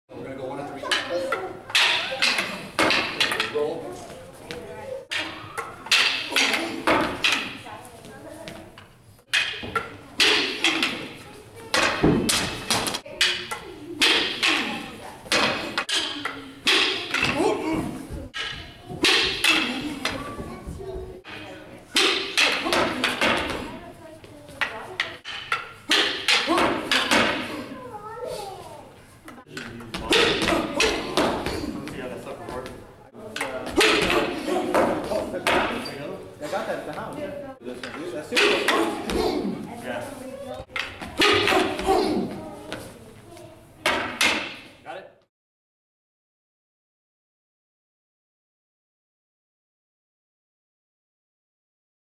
Stockton Multi-Style Escrima / Espada Y Daga - June Gotico
Video of Stockton Multi-Style Instructor June Gotico practicing Espada Y Daga (Sword & Dagger) techniques.